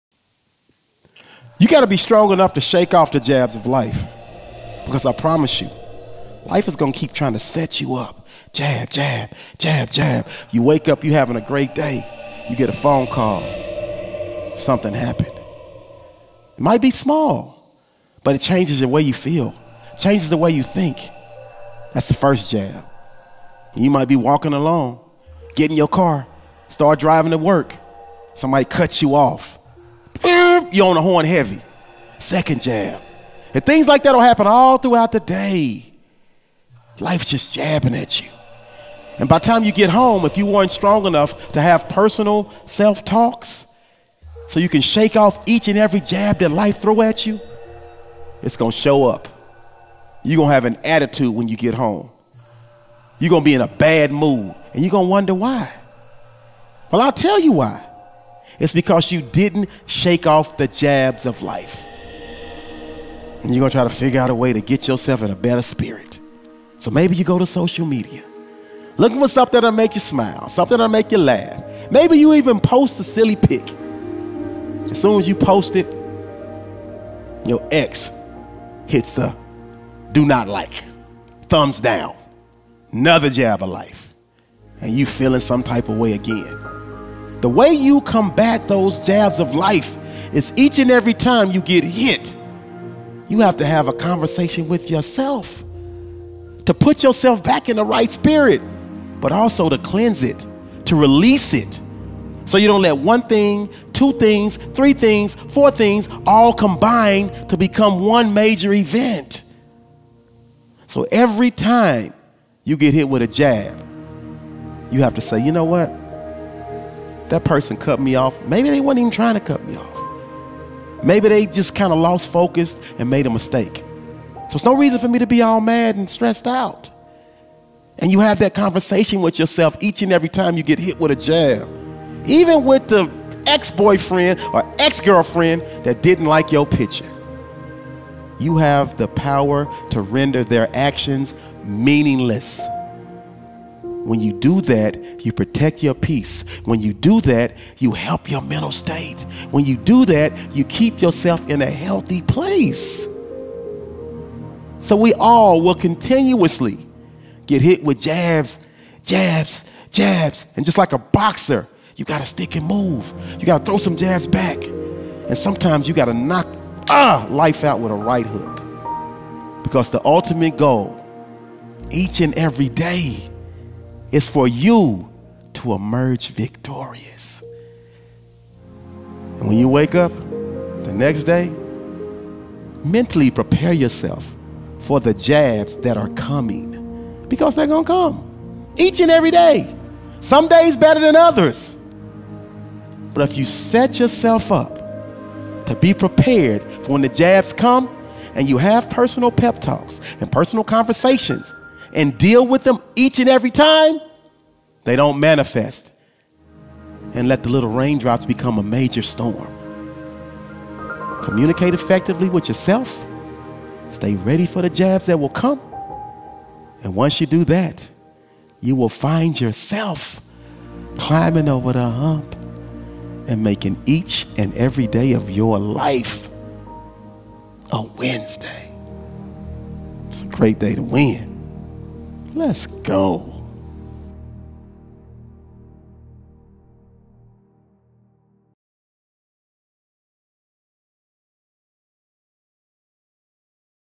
How To Overcome Challenges In Life | Daily Motivation Dose | Freddy Fri
In this video, I want to help overcome any challenges you may be facing or dealing with in life.  My name is Freddy Fri and this is Freddy Fri Motivation!

This episode is designed to help you overcome any challenges you may face in your life, you really have to be strong enough to shake off those jabs of life, because I promise you life is going to keep trying to set you up.

If you don't know how to have personal self-talks with yourself when you are dealing with these jabs they will be with you all day, and by the time you get home, you will have that attitude that no one likes once you get home, you will be in a bad mood and you will wonder why.

The only way you can avoid those jabs at life is by making sure that each and every time you receive a jab you sit down with yourself, have a conversation with yourself, to put yourself back in the right spirit.